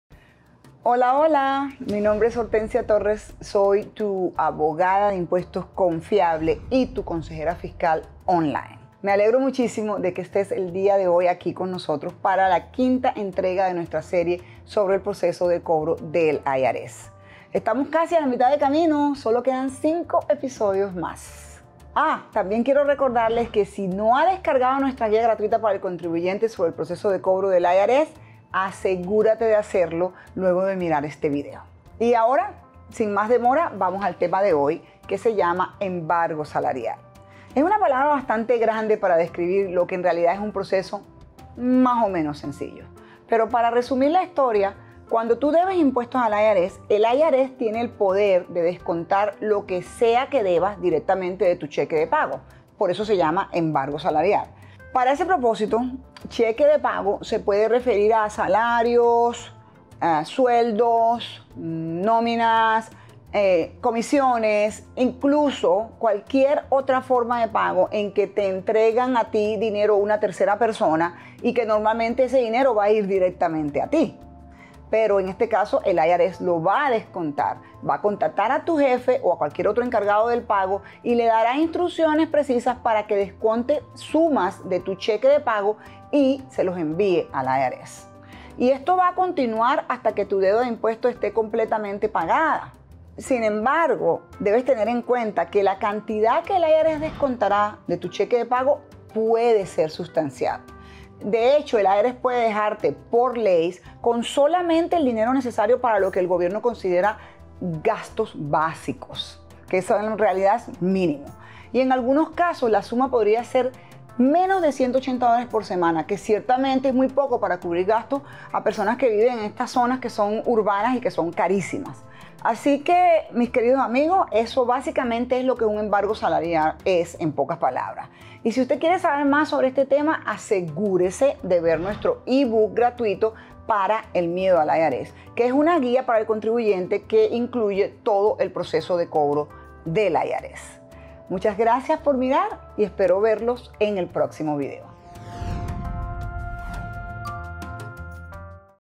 Qué son los embargos salariales
¿Le debe dinero al IRS? ¿Busca una resolución de deuda tributaria? ¡Entonces este video es para tí!

Este es el quinto de una serie de diez videos sobre el proceso de cobranza del IRS.

En este quinto episodio, la abogada fiscal Hortencia Torres explica Qué son los embargos salariales.

Este video complementa nuestra guía gratuita llamada "PARA EL MIEDO AL IRS: una guía completa sobre el proceso de colección del Departamento de Renta Interna ".